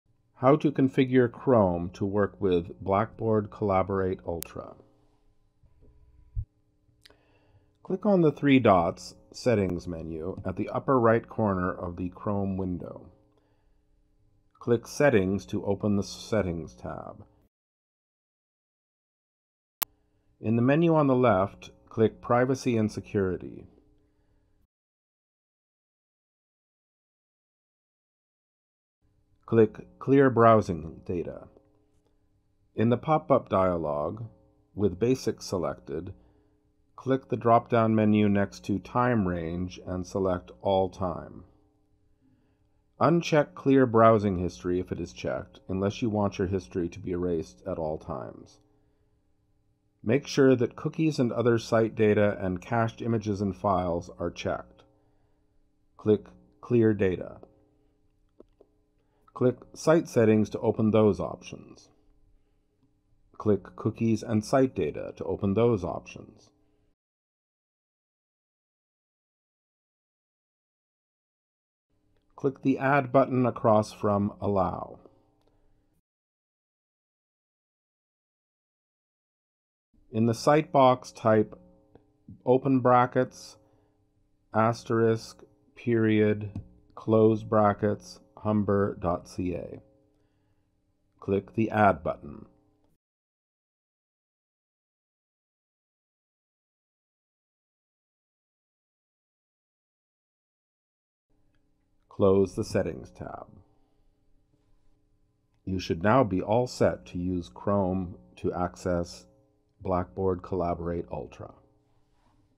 How to configure Chrome to work with Blackboard Collaborate Ultra
Video tutorial by Naveed Aqeel